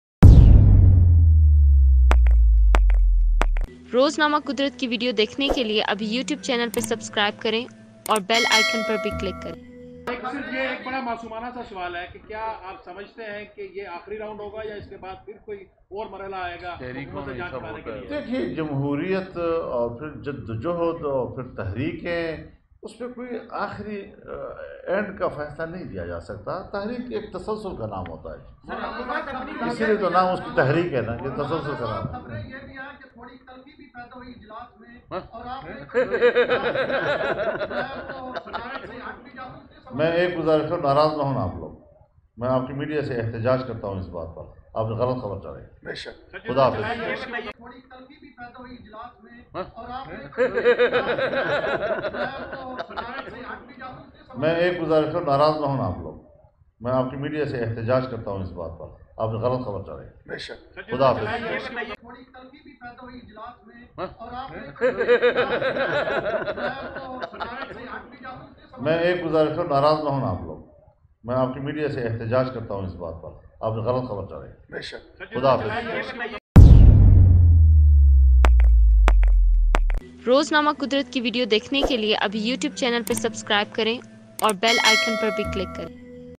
PDM Maulana Fazal ur Rehman Angry on Media | ناراض نہ ہونا؟مولانافضل الرحمٰن خود ناراض ہوگئے
صحافی کے سوال میڈیا کو کھری کھری سنادی‘پریس کانفرنس چھوڑ کر چلے گئے